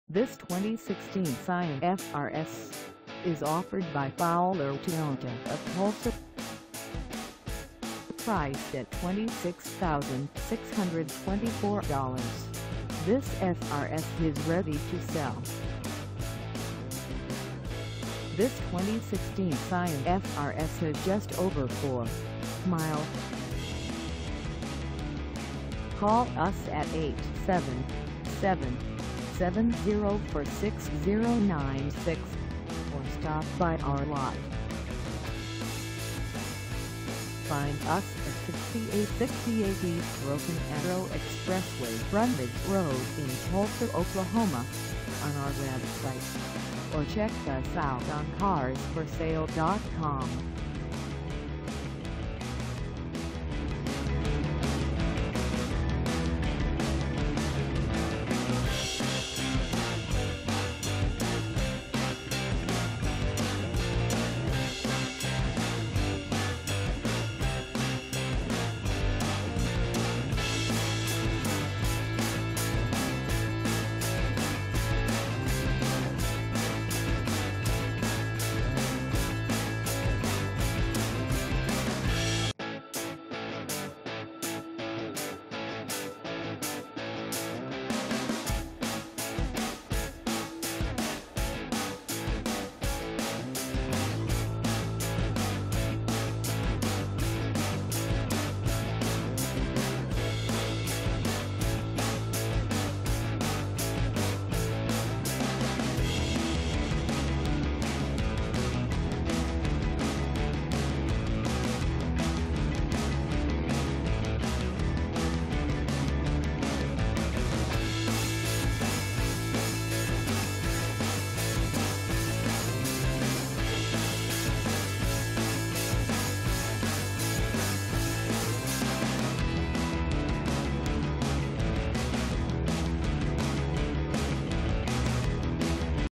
2016 Scion FR-S Base 2dr Coupe 6A for sale in Tulsa, OK 7414
This 2016 Scion FR-S Base 2dr Coupe 6A is for sale in Tulsa, OK 74145 at Fowler Toyota of Tulsa.

Engine: 2.0L H4
Transmission: Automatic 6-Speed
Color: Raven
Mileage: 4
Contact Fowler Toyota of Tulsa 6868 E Broken Arrow Expressway Frontage Rd Tulsa, OK 74145 to test drive this 2016 Scion FR-S Base 2dr Coupe 6A today.